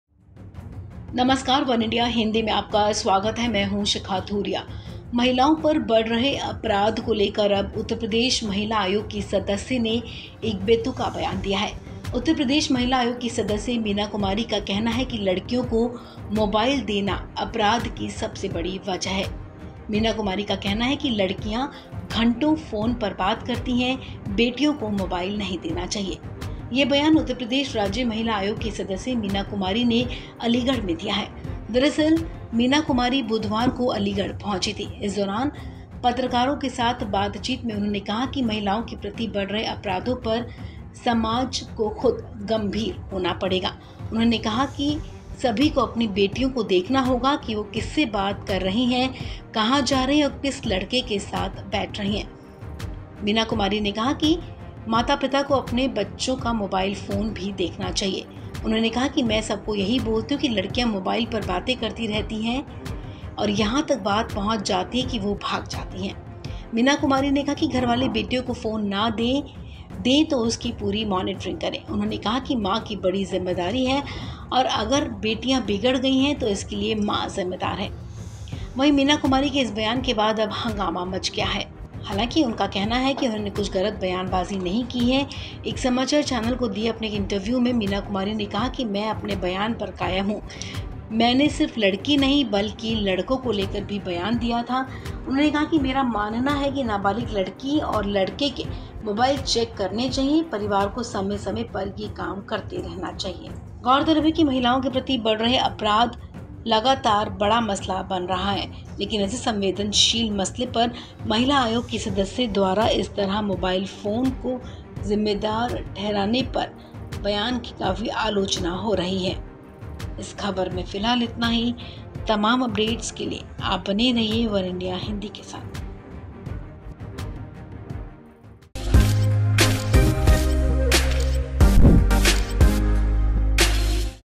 UP Women Commission member Meena Kumari का बयान, लड़कियों को ना दें मोबाइल | वनइंडिया हिंदी
Now a member of the Uttar Pradesh Women's Commission has given an absurd statement regarding the increasing crime against women, Meena Kumari, a member of the Uttar Pradesh Women's Commission, says that giving mobiles to girls is a big reason for crime.

महिलाओं पर बढ़ रहे अपराध को लेकर अब उ Uttar Pradesh Women's Commission की सदस्य ने एक बेतुका बयान दिया है,  Uttar Pradesh Women's Commission Meena Kumari का कहना है कि लड़कियों को मोबाइल देना अपराध की बड़ी वजह है।